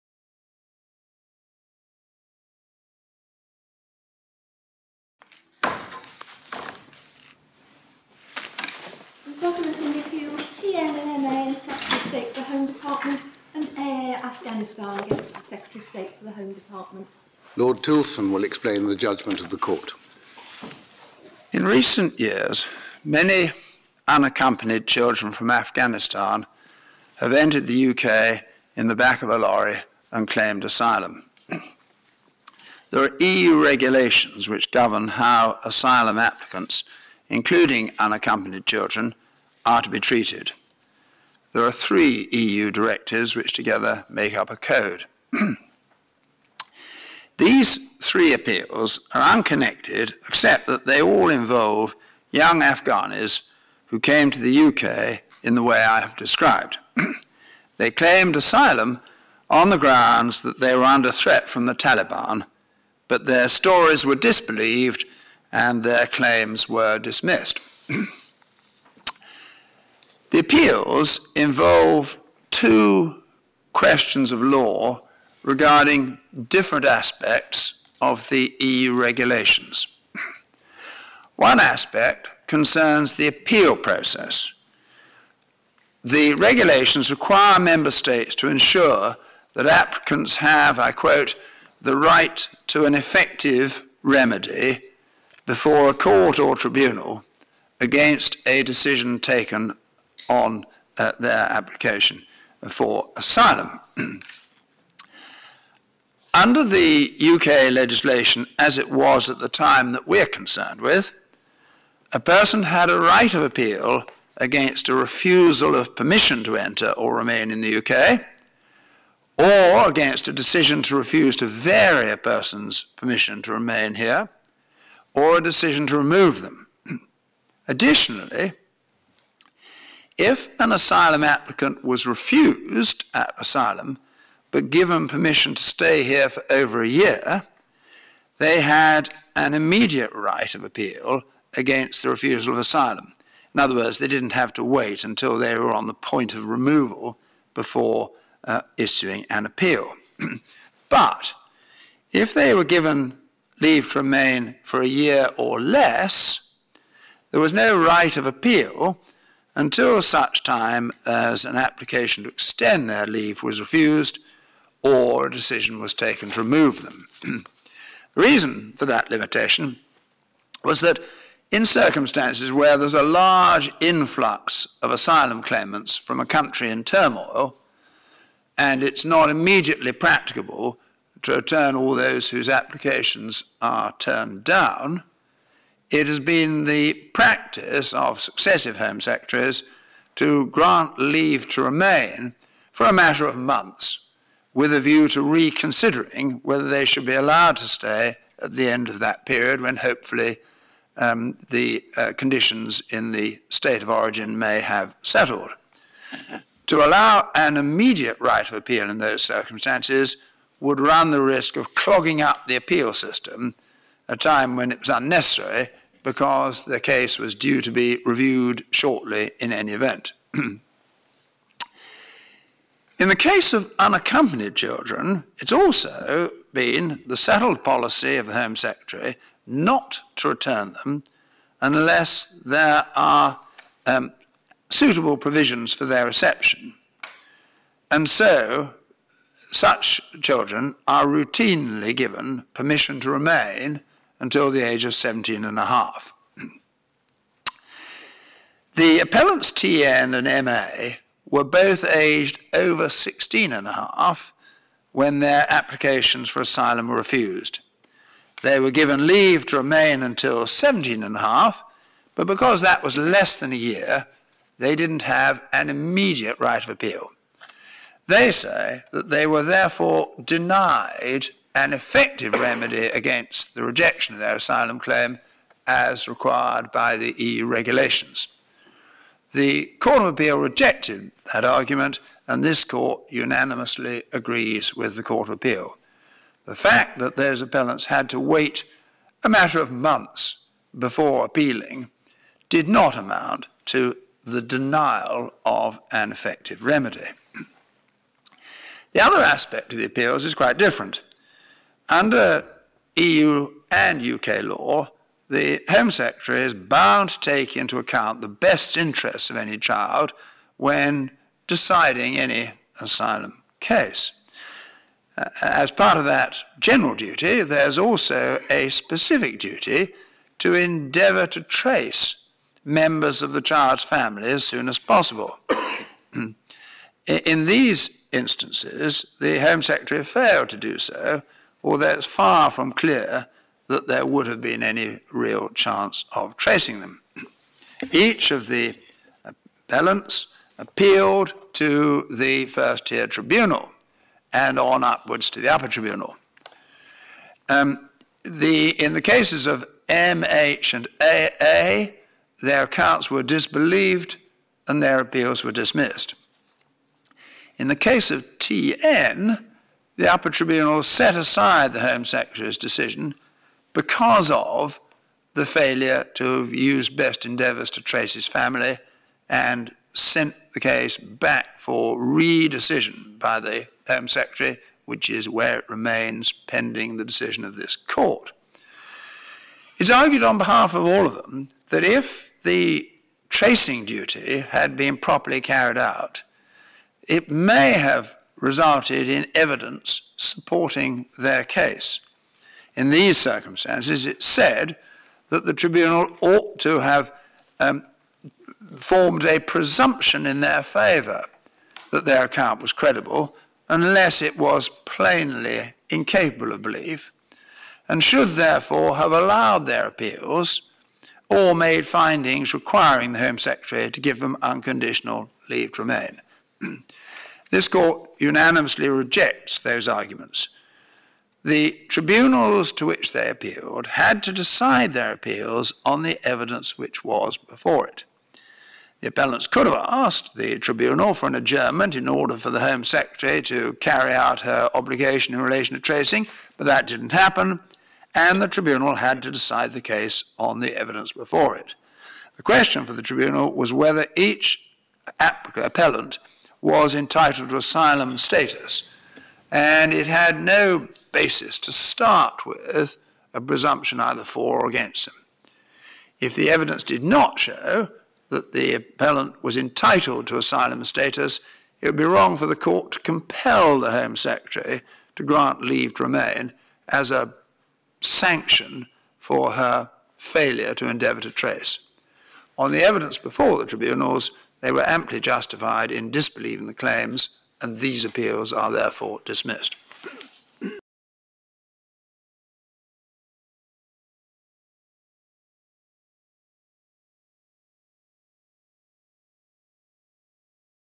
TN, MA & AA (Afghanistan) v Secretary of State for the Home Department
UKSC 2014/0047
TN and MA (Afghanistan) (AP) (Appellants) v Secretary of State for the Home Department (Respondent) UKSC 2014/0047

UKSC 2014/0081
AA (Afghanistan) (Appellant) v Secretary of State for the Home Department (Respondent)
On appeal from the Court of Appeal (Civil Division) (England and Wales)

TN, MA and AA are Afghan nationals who came to the UK as unaccompanied minors. They claimed asylum and had their accounts disbelieved and their claims rejected.
In the case of TN and MA, they were originally granted discretionary leave to remain (DLR) here till they reached 17½, which was less than a year. Consequently, they were not entitled to a statutory appeal against the Home Secretary’s decision to refuse their asylum claims because the right to an appeal arises after DLR of one year. But they were able to seek judicial review of it. This appeal considers whether the ability to judicially review the Home Secretary’s refusal to grant them asylum is an effective remedy for the purposes of EU law.
In the case of AA, he argues that the Secretary of State had a duty to try to trace his family. She made no effort to do so, while also refusing his application for asylum. The Court of Appeal found that the breach nonetheless made no difference to AA’s claim for asylum, as he had not given her any information that she could use to trace his family. This appeal considers extent of the Secretary of State’s duty to try to trace the families of unaccompanied children who seek asylum in Britain and what follows if it is breached.

The Supreme Court unanimously dismisses the appeals.